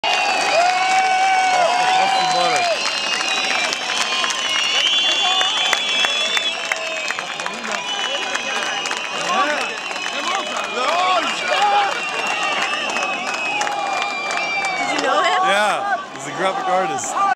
Ganzeer - Tahrir Square, Jan25, 2011
Rachel Beth Anderson captures Ganzeer spraying a billboard in Tahrir Square on Jan 25, 2011